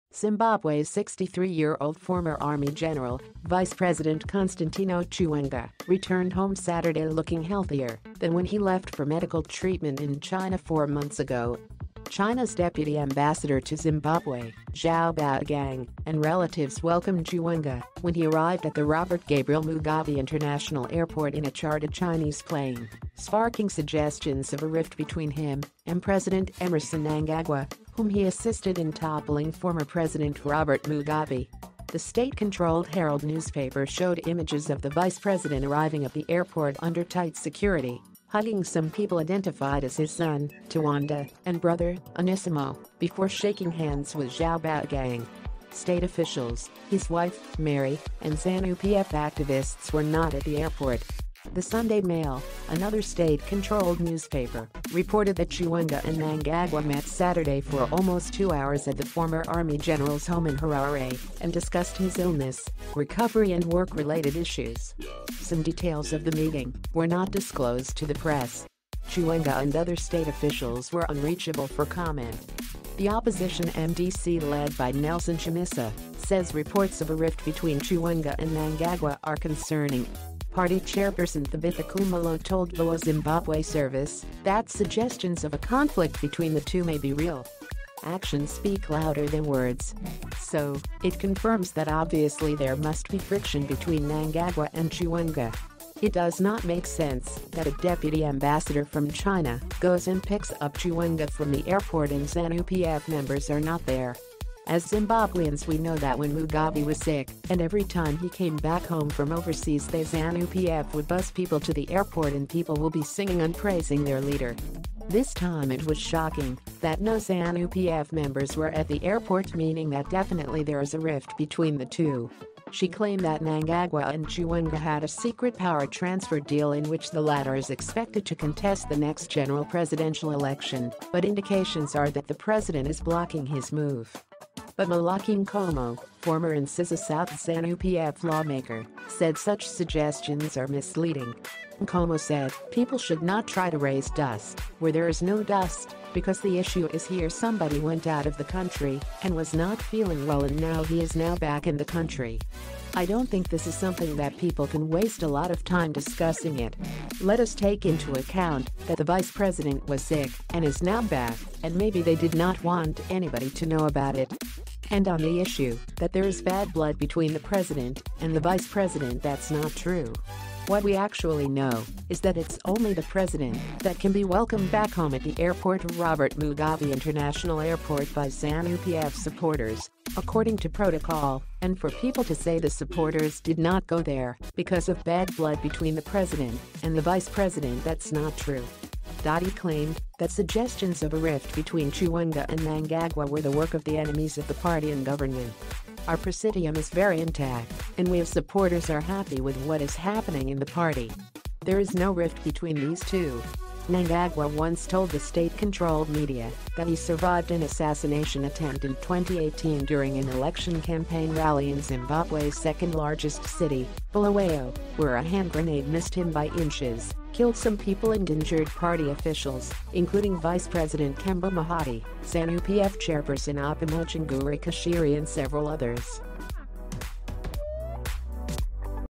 Zimbabweans Worried Over Alleged Chiwenga, Mnangagwa Rift
Zimbabwe’s 63-year-old former army general, Vice President Constantino Chiwenga, returned home Saturday looking healthier than when he left for medical treatment in China four months ago.